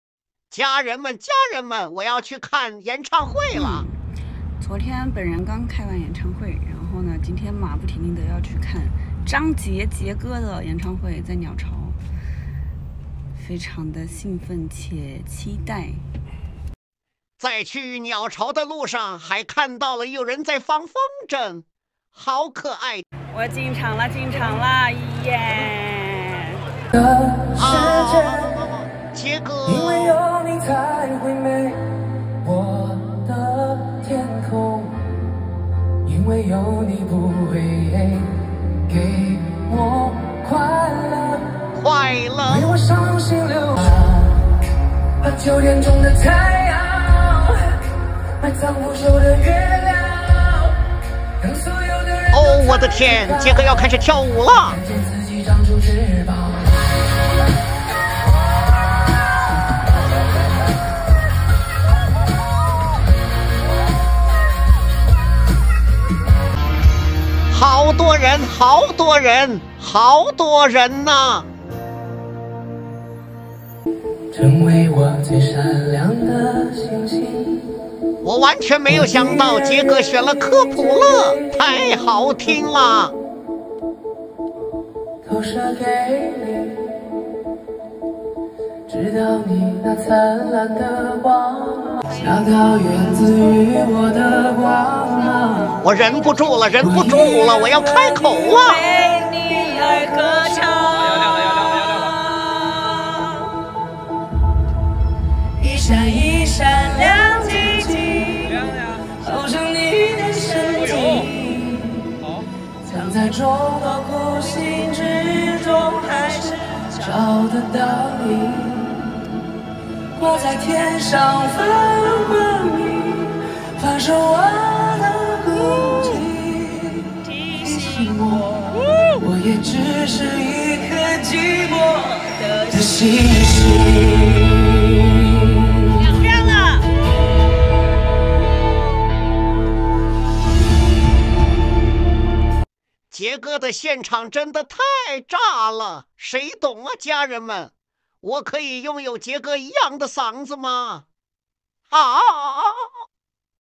孟佳抖音 Meng Jia's TikTok 20230827
杰哥真的 唱的太好了
学到了好多
坐在第一排 杰哥的声音直接贯穿我的颅顶
太牛了！！！！！！！  ​​​

Cr：M孟佳J微博